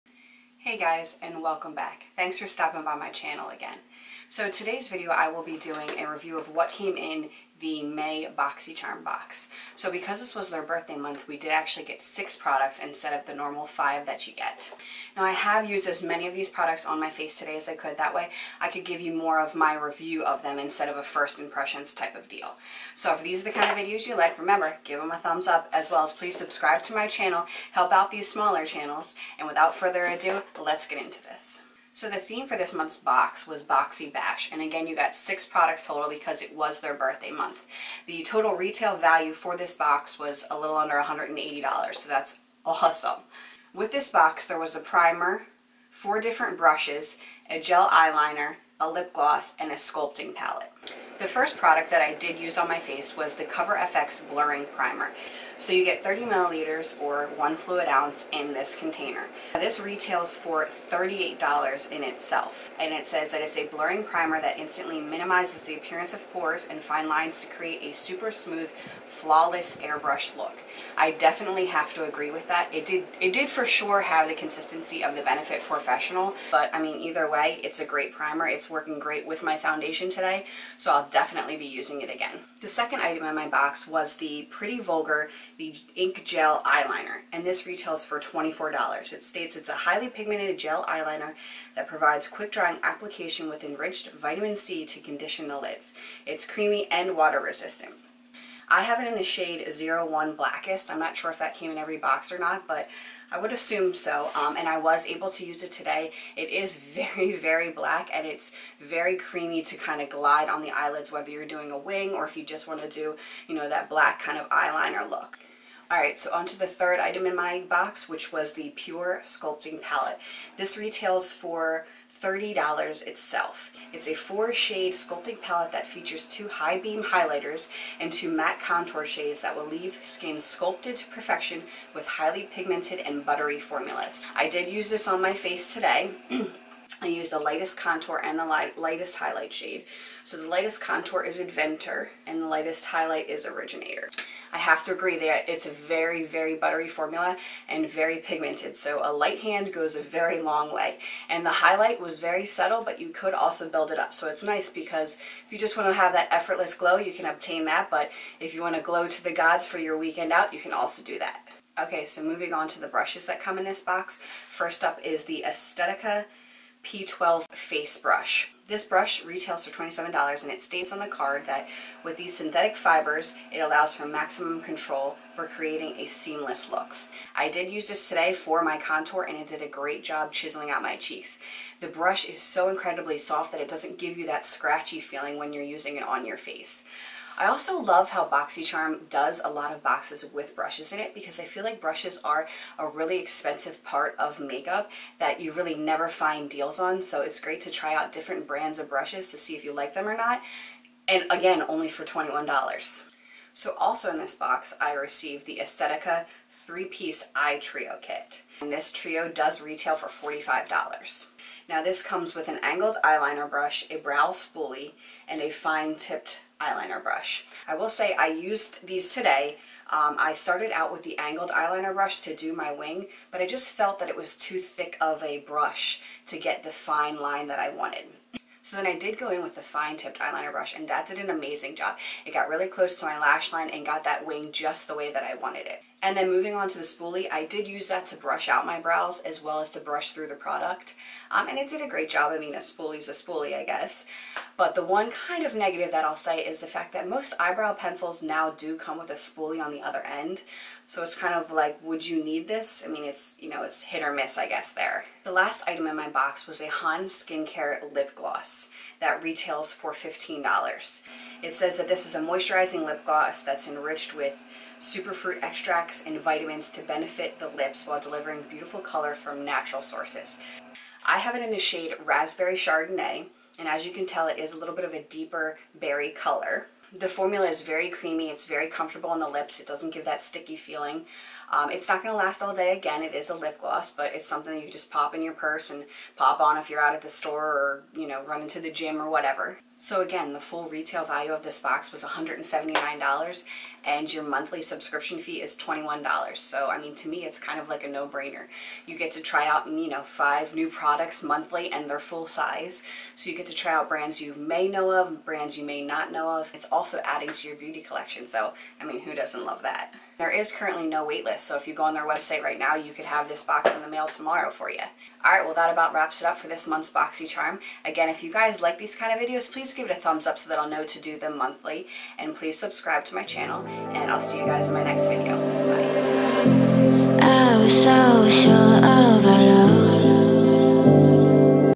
May 2018 BoxyCharm Unboxing/Reivew
Hey guys! Welcome back! In today's video I unbox and review the contents of the May 2018 Boxycharm subscription. If you are wondering if the box is worth the monhly subscription, then tune in!